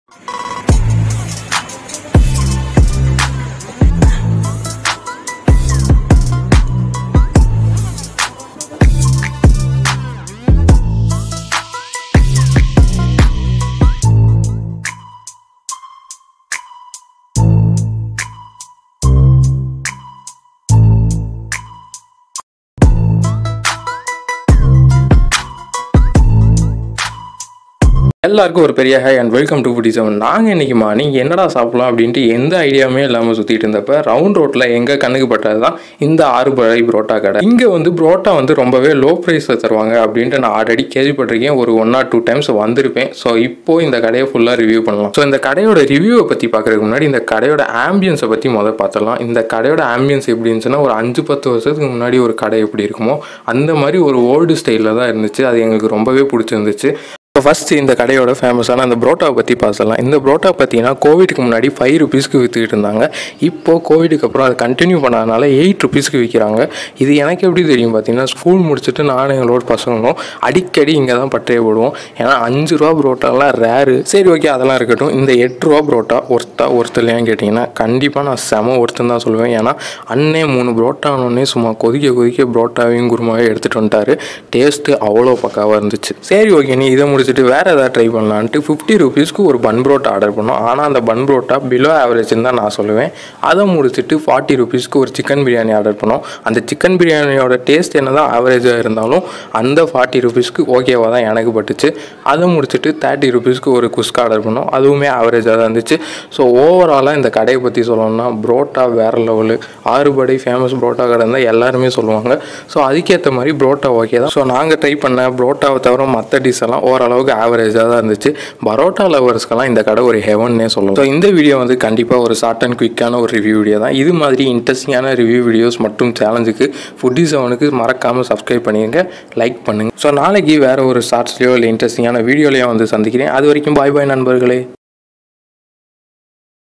₹8Rs famous parotta kadai in dindigul | Foodies heaven
₹8 Rupees famous parotta kadai in dindigul

Location :
AARUPADAI PAROTTA KADAI,near round road, dindigul

Prices :
Parotta : 8Rs
Kuska : 30Rs
Chicken biryani : 40Rs
Kalakki : 15Rs